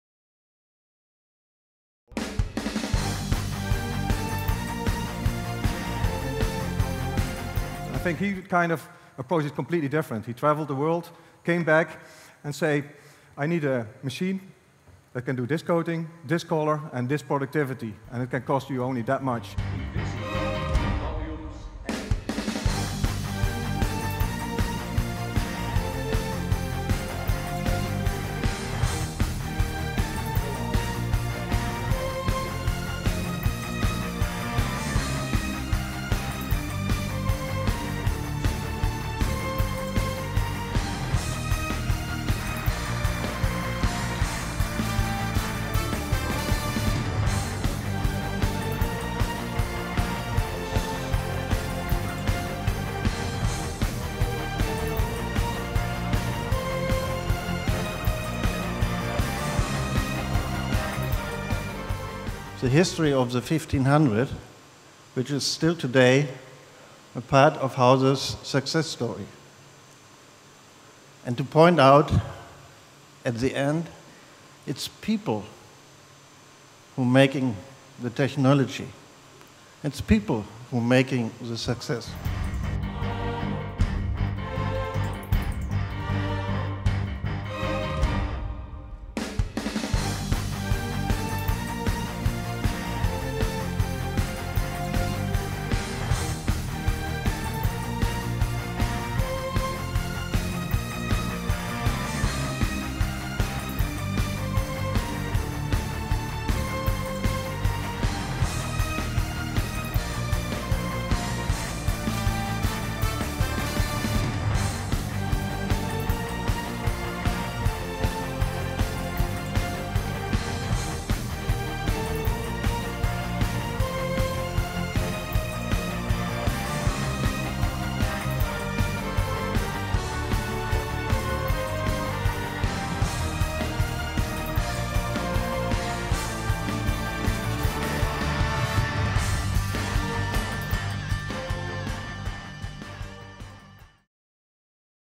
Hauzer Metalliner Launch
In March 2012, Hauzer hosted the conference "Environment protection by plasma coating technology" with speakers from well-known companies such as BMW, University of Leeds and Fraunhofer. Visitors from all over the world attended. At the same time the Hauzer Metalliner, a flexible inline production platform, was launched.